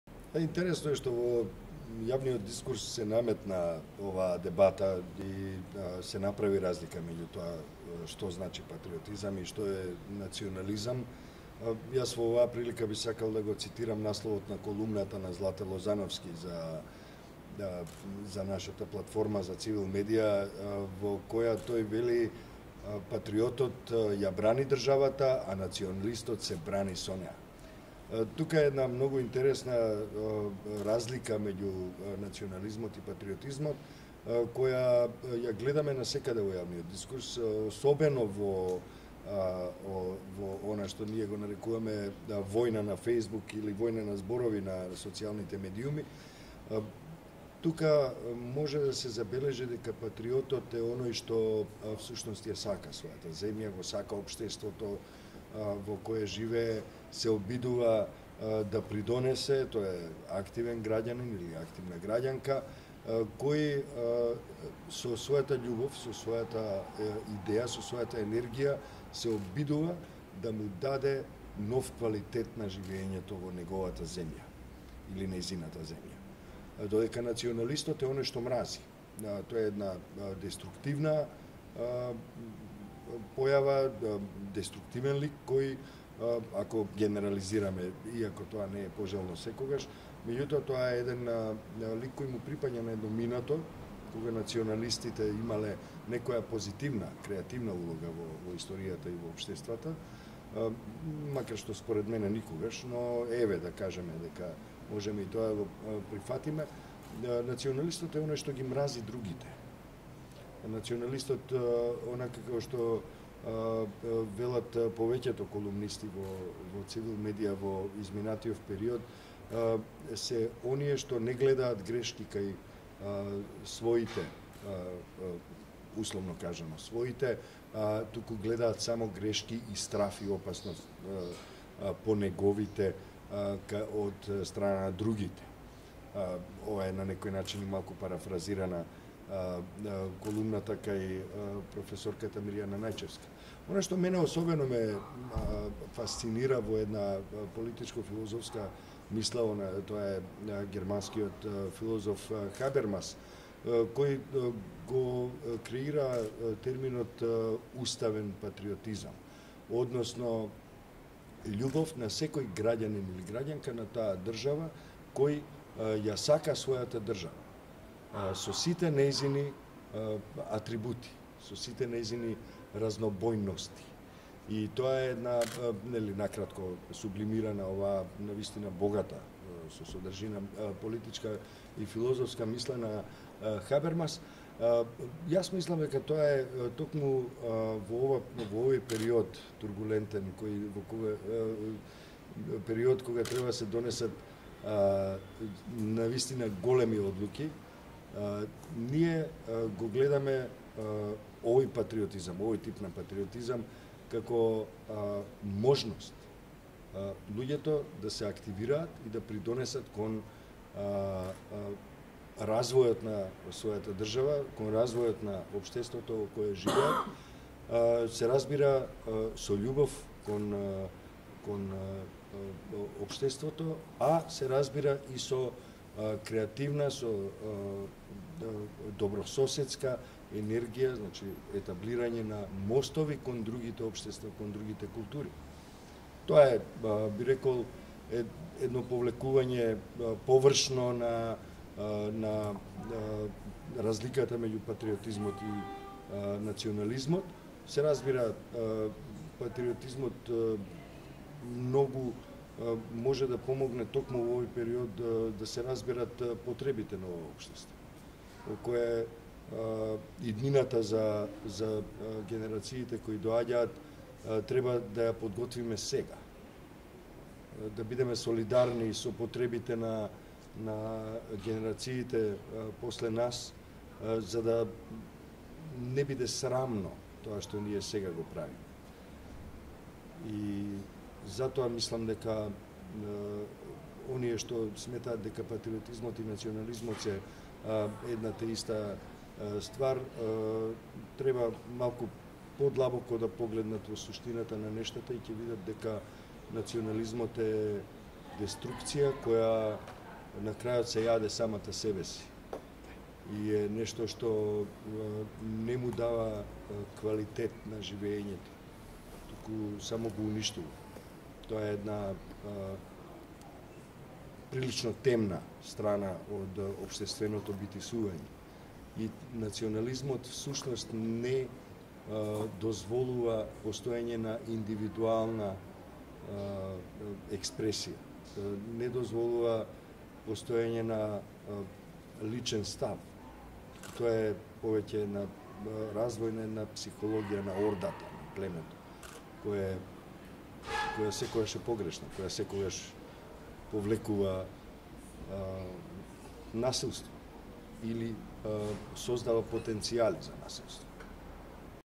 Дерала: Национализмот е деструкција која се јаде самата себе си
Дерала, ЦИВИЛ: Интересно е што во јавниот дискурс се разви оваа дебата и се направи разлика помеѓу тоа што значи национализам, а што патриотизам. Јас во оваа прилика би сакал да го цитирам насловот на колумната на Злате Лозановски, напишана за нашата платформа – ЦИВИЛ Медиа, во која тој вели: „Патриотот ја брани државата, а националистот се брани со неа!“.